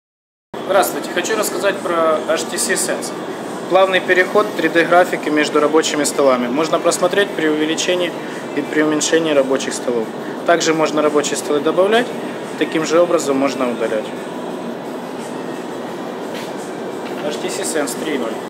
HTC Sense 3.0
Демонстрация HTC Sense 3.0  на телефоне HTC One X.